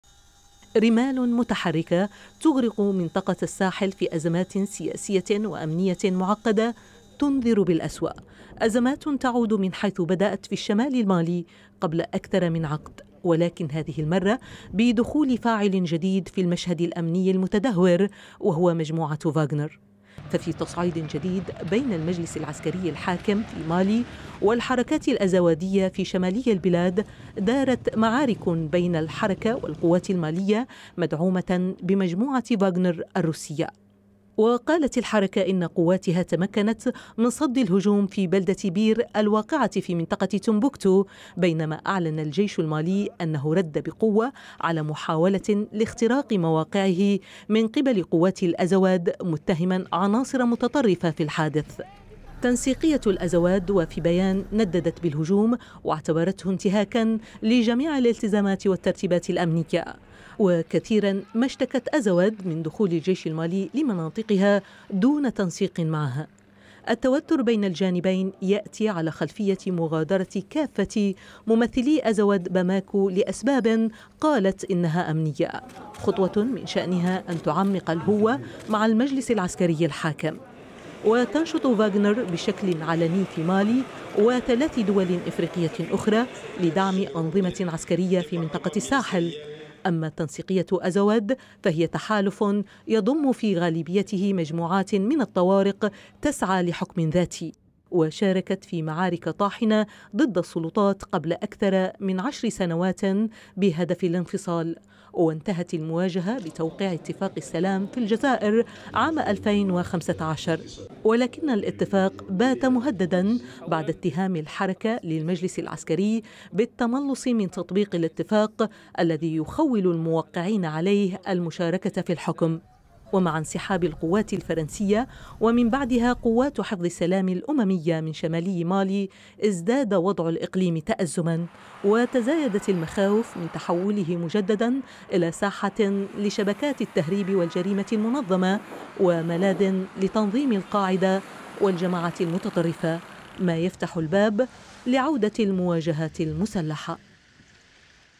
حركة أزواد تتهم فاغنر والجيش المالي بمهاجمة مواقعها شمالي البلاد
اتهمت حركة أزواد الجيش المالي ومجموعة فاغنر الروسية بمهاجمة مواقعها في منطقة تمبكتو شمالي البلاد.
وقالت الحركة في بيان إنها صدت الهجوم ووصفته بأنه انتهاك لجميع الالتزامات والترتيبات الأمنية